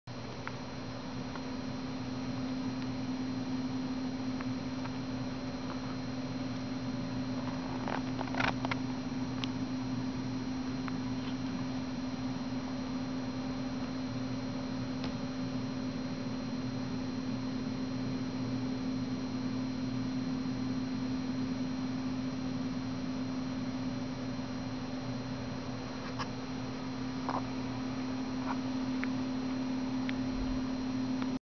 Dancerina close up
This working Dancerina doll will be listed on eBay starting Dec 14 2011 at 6PM EST.  The eBay number is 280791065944